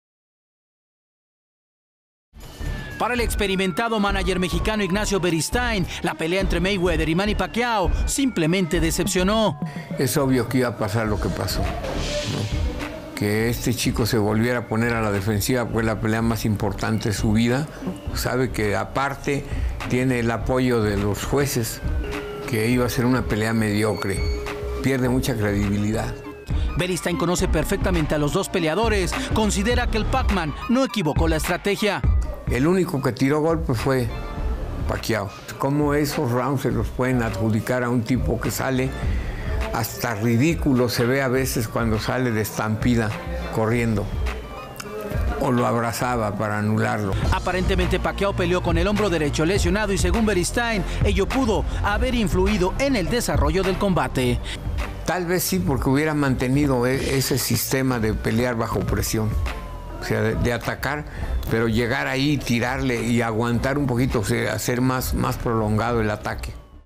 Nacho Beristain Reaction on Mayweather vs Pacquiao (English Subs) - Artorias Boxing
Nacho always keeping it real. If you don't know Nacho, he even kept it real about some dark moments on his career as coach. Like one time when José Sulaimán heard him giving bad recommendations to his fighter and disproved in disjointed shaking his head. It was a nice anecdote.